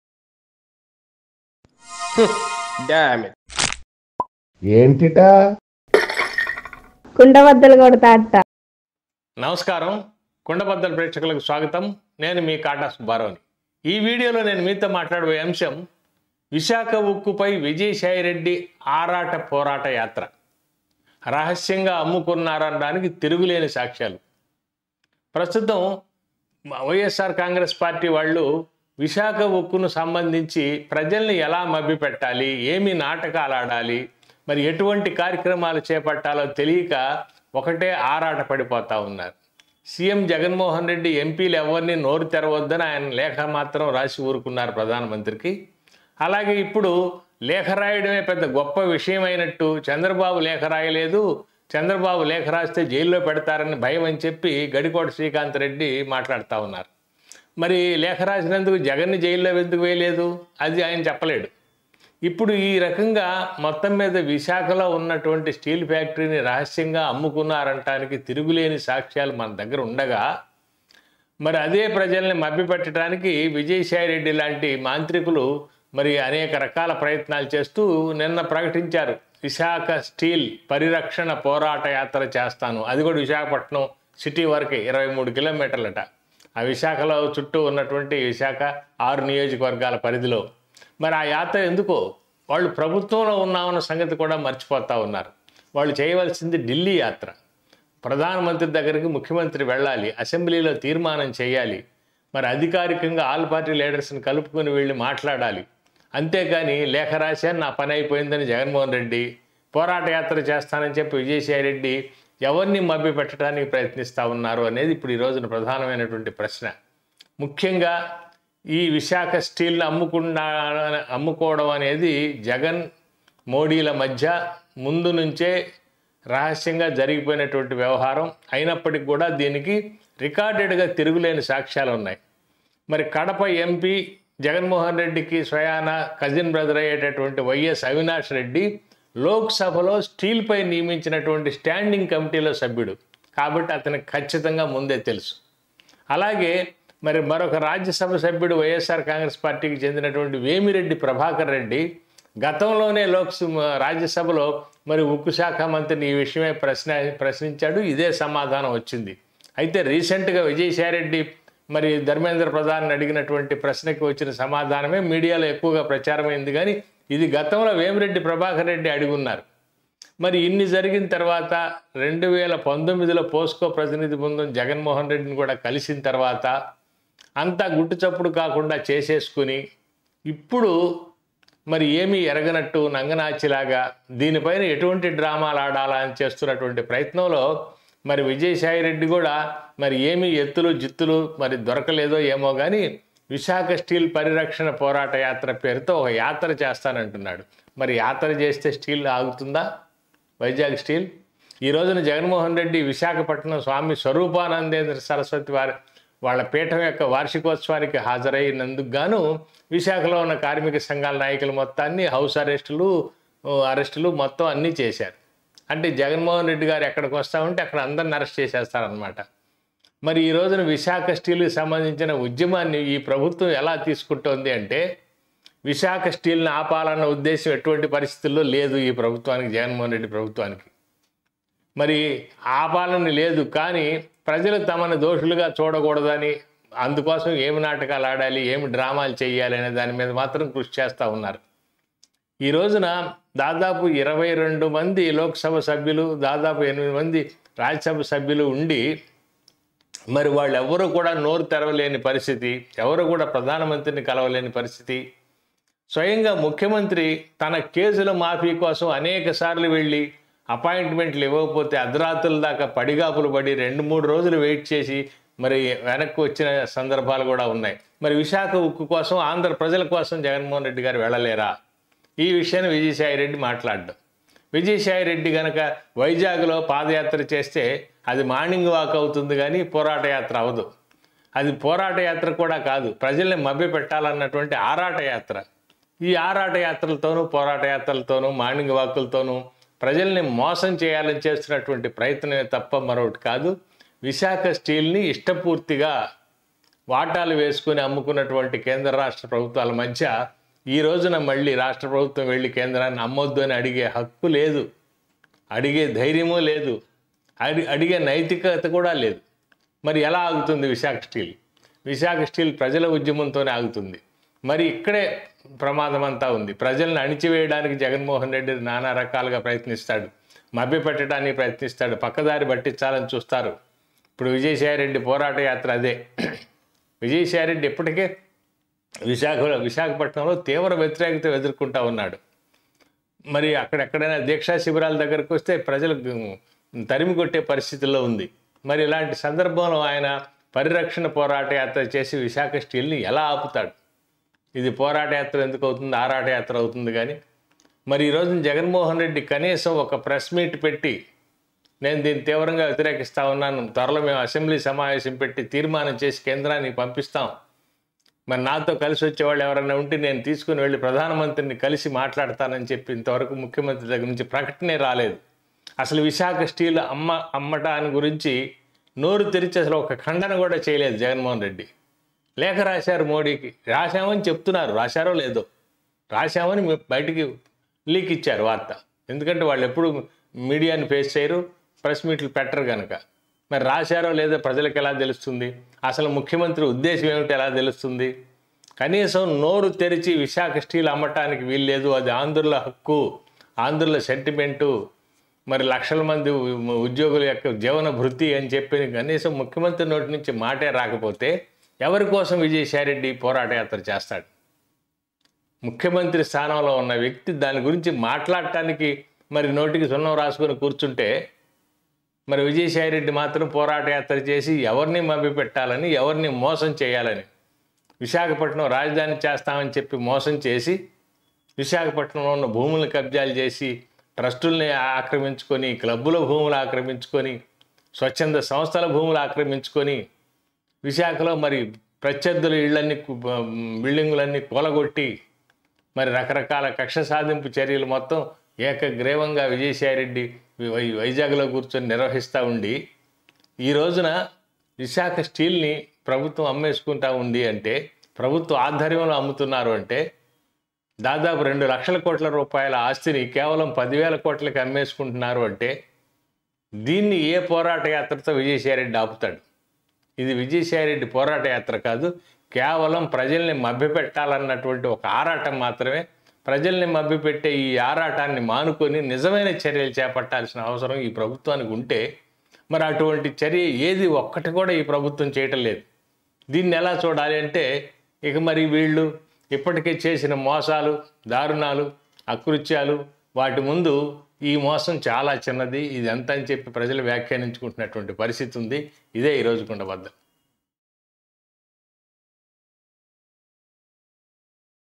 ఉక్కు పై విజయసాయిరెడ్డి ఆరాట పోరాట యాత్ర | అమ్మకంలో YS జగన్ పాత్రపై తిరుగులేని సాక్ష్యాలు
Vijayasaireddy,
Kundabaddalu,
Vizag Steel Plant,
Vemi Reddy,